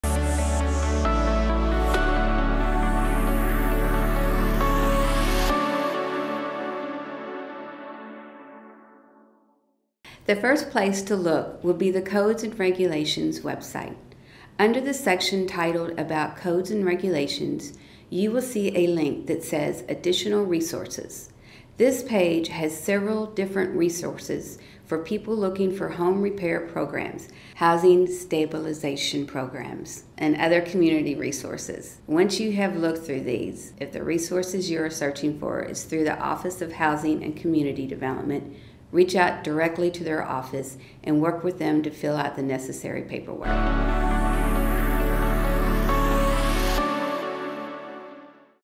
Home Improvement Resources | Codes and Regulations
Code Enforcement Supervisor, Tammy Goatley explains the home improvement resources that Louisville Metro provides.